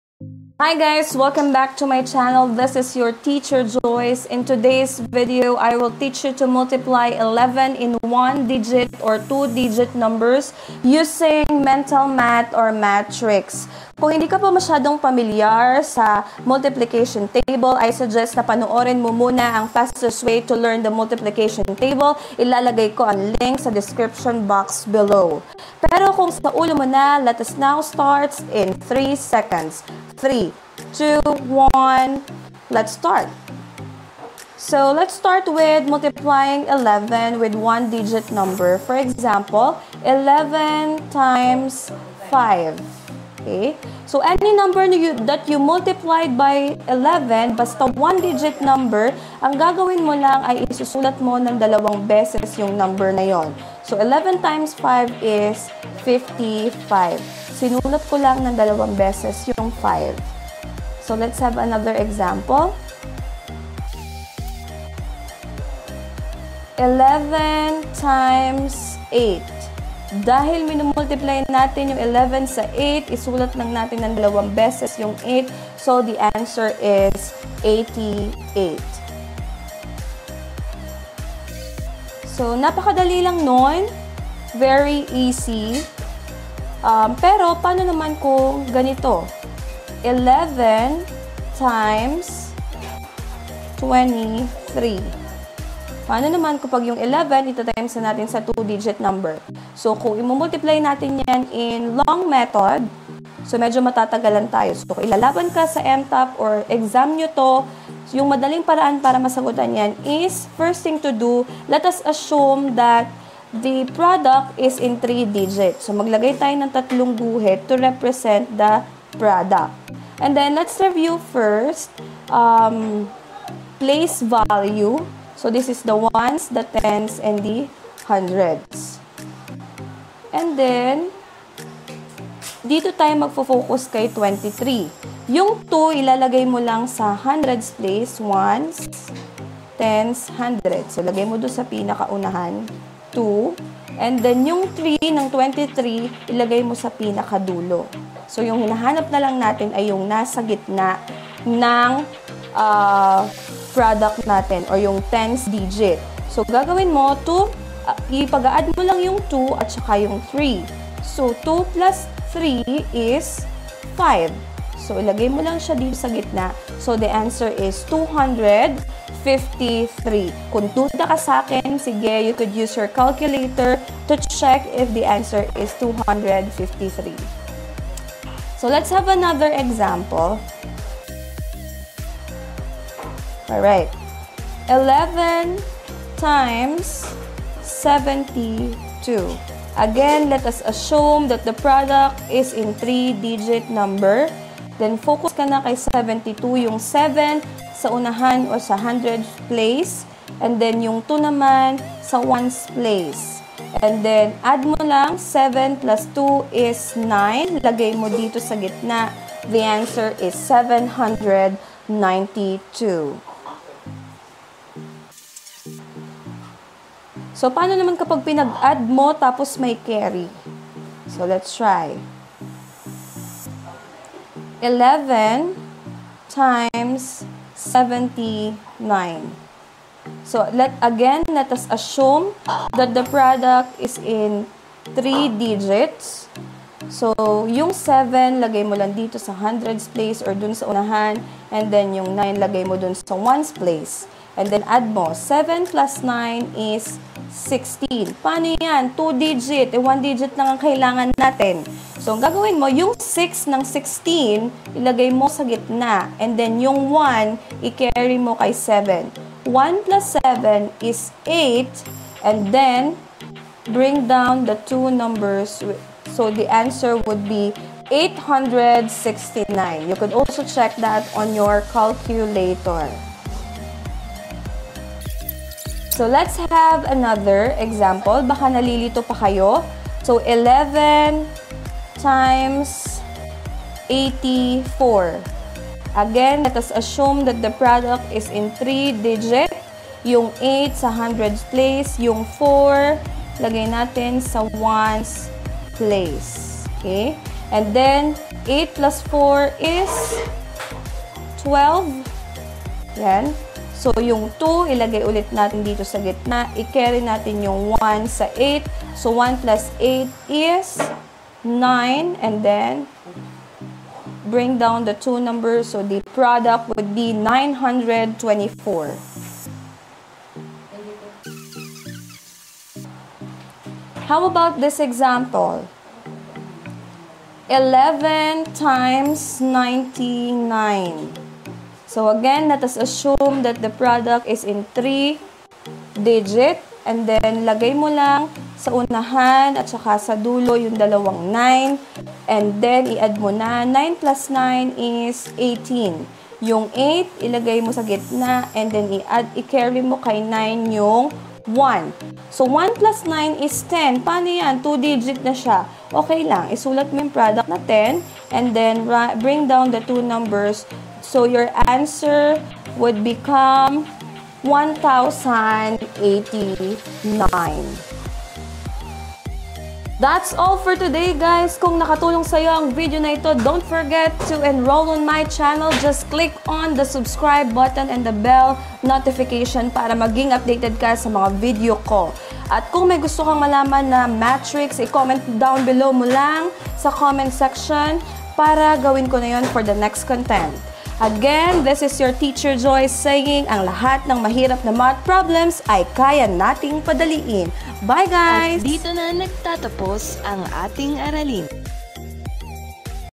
TIMES TABLE TRICK (ELEVEN): MENTAL MULTIPLICATION MATH TRICKS
This video will help you to multiply 11 in 1 digit and 2 digit numbers using mental math tricks! Solve multiplication problems  as fast as 5 seconds!

Kung nahihirapan ka magmemorize ng multiplication table, watch this video ang master  it in 10 minutes!